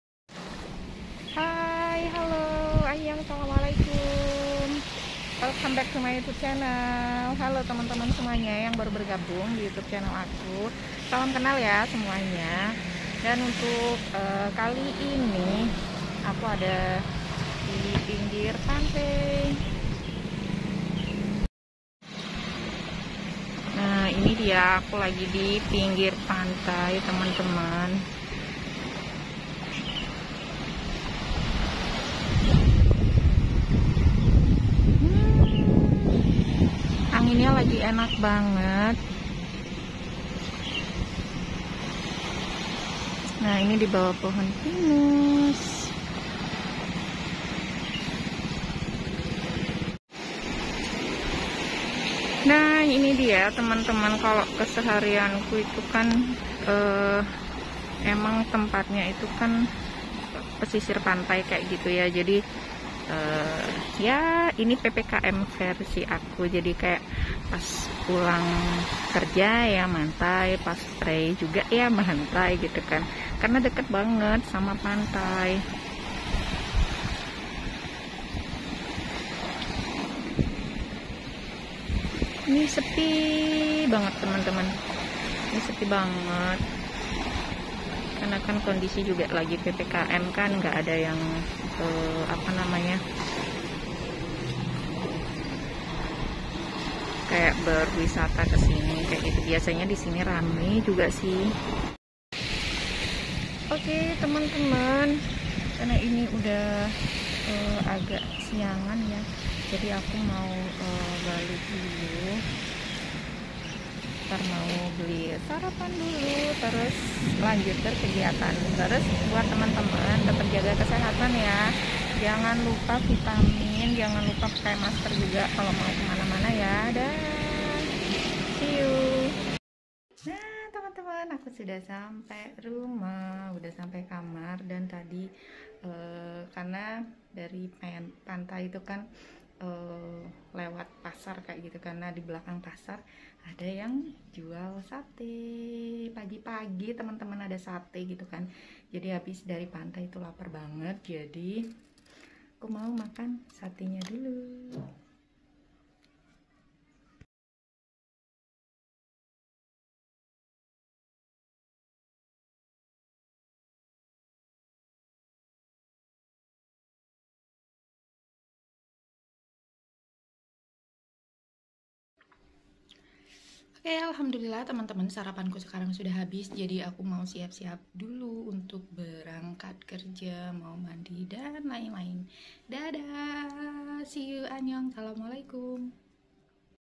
Pagi Pagi Sarapan Sate Lontong, PPKM Pas Prei Kerjo Mantai Rek
Sarapan Sate Pagi pagi di Bali
PPKM semua jadi sepi ya termasuk pantai. Habits mantai eh ketemu Dagang Sate, Tumben2an ketemu Dagang Sate yang jualan dari pagi Pagi banget.
PPKM versi aku pas pulang kerja mantai, mau mantai kapan saja bisa karena emang dikelilingi laut kannnn